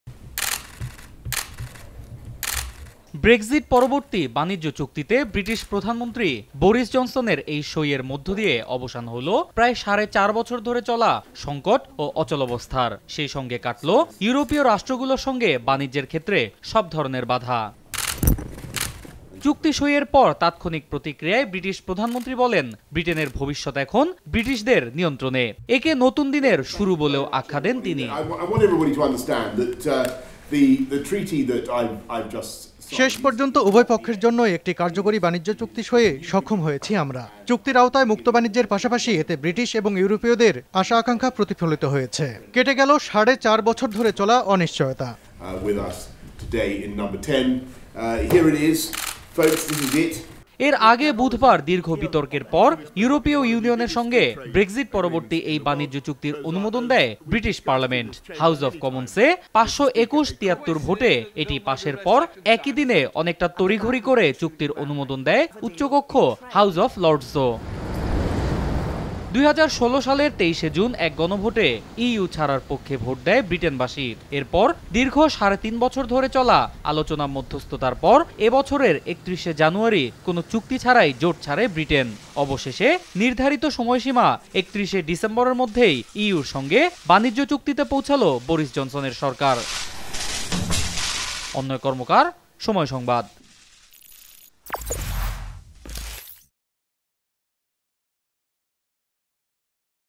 অবসান হলো সাড়ে ৪ বছরের সংকট ও অচলাবস্থার | Brexit | UK News
অবশেষে ব্রেক্সিট পরবর্তী বাণিজ্য চুক্তিতে সই করে একে আইনে পরিণত করলেন ব্রিটিশ প্রধানমন্ত্রী বরিস জনসন। বৃহস্পতিবার স্থানীয় সময় রাত ১১টা থেকে এই চুক্তি কার্যকর হবে বলে জানা গেছে। সইয়ের পর এক তাৎক্ষণিক প্রতিক্রিয়ায় ব্রিটিশ প্রধানমন্ত্রী বলেন, এই বাণিজ্য চুক্তির মধ্যে দিয়ে ব্রিটেনের ভবিষ্যত ব্রিটিশদের নিয়ন্ত্রণে এলো